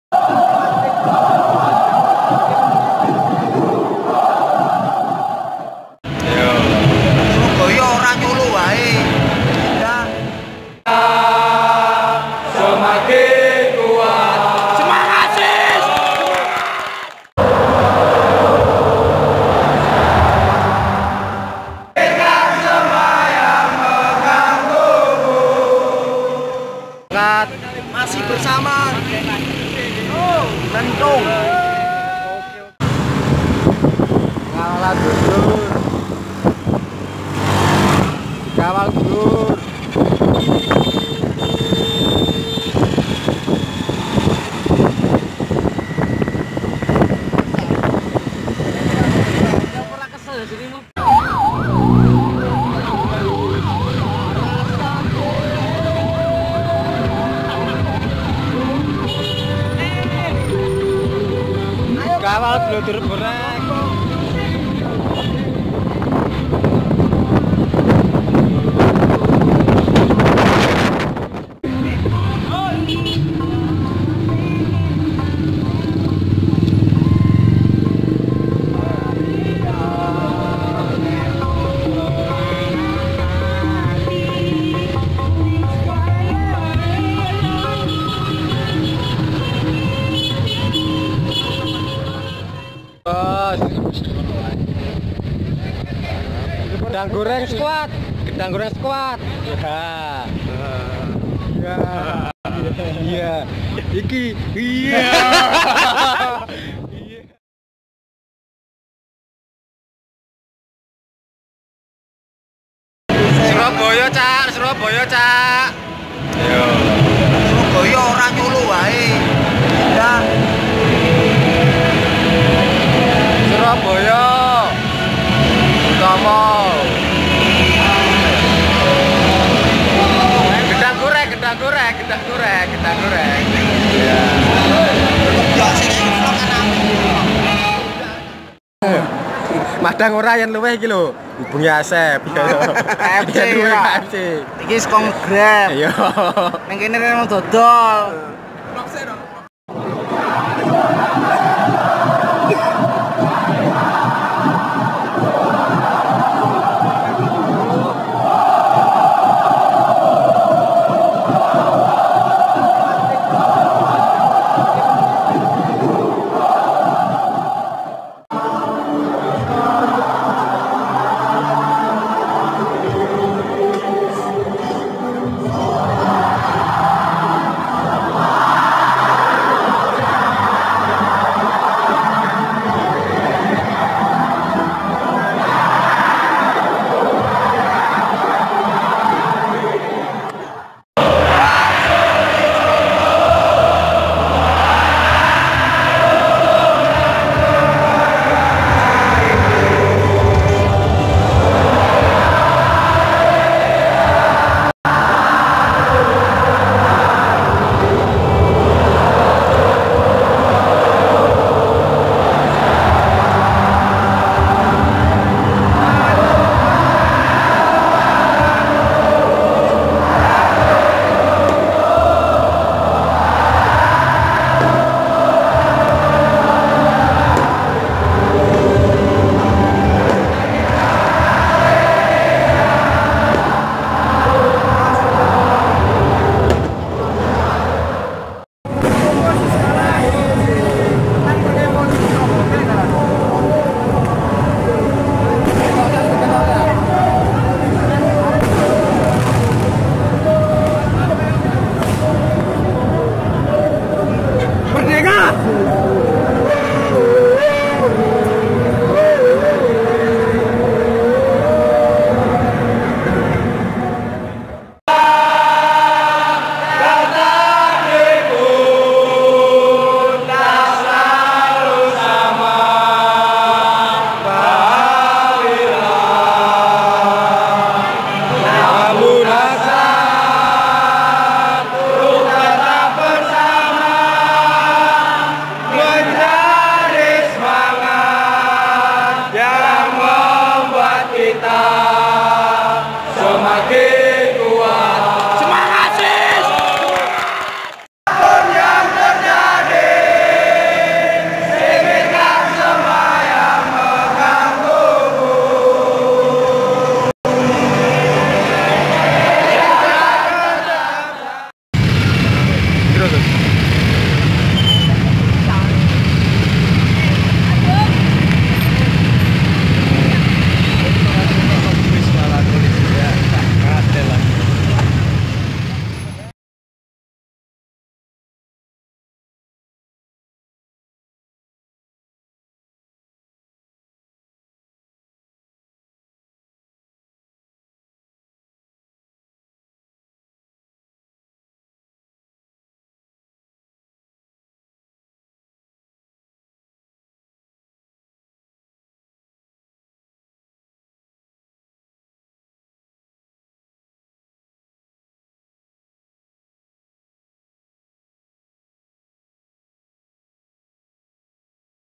BONEK sambut SOLO fans Di Surabaya ✔ PERSEBAYA VS PERSIS SOLO
Sebelas Januari bertemu di Surabaya
Sambutan baik BONEK atas kedatangan supporter PERSIS SOLO di stadion Gelora Bung Tomo.
pengawalan bonek dari masuk kota surabaya sampai masuk stadion,

untuk bonek dimana pun berada, terimakasih atas sambutannya
Surabaya 11 Januari 2020